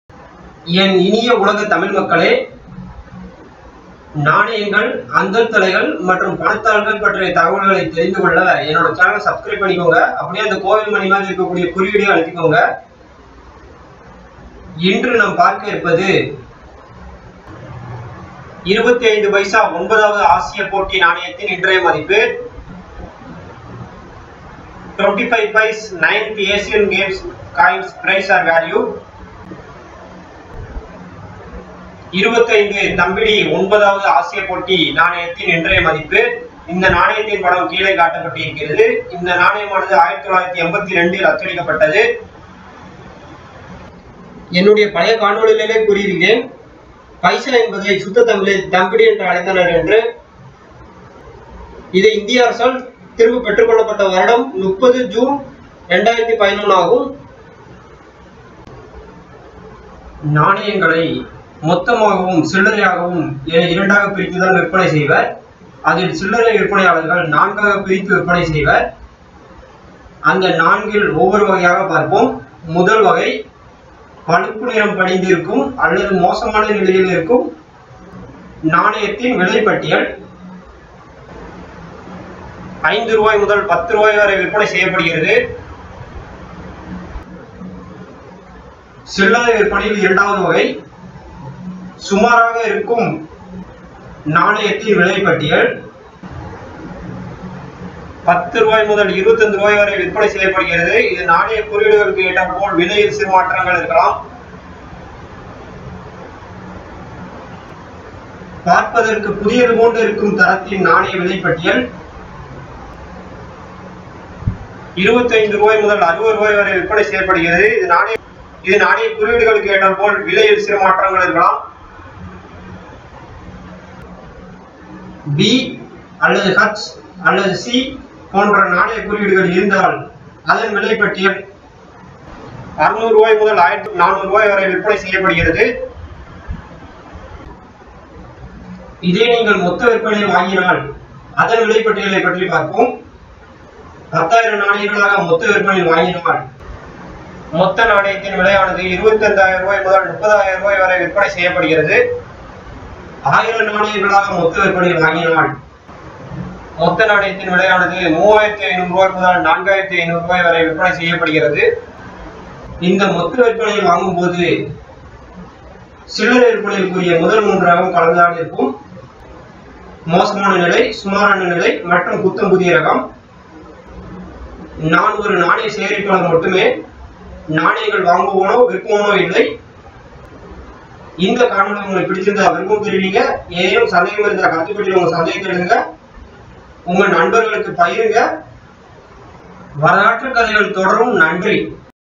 25 Paise IX Asian Games Coins Price|| 25 பைசா ஒன்பதாவது ஆசிய போட்டி நாணயத்தின் இன்றைய மதிப்பு
இருபத்து ஐந்து பைசா ஒன்பதாவது ஆசிய போட்டி  நாணயத்தின் இன்றைய மதிப்பு அடங்கிய காணொளி

                        ஏதேனும் சந்தேகம் இருந்தால் கீழ்க்காணும் முகநூல் மற்றும் டுவிட்டரில் தொடர்பு கொள்ளலாம்.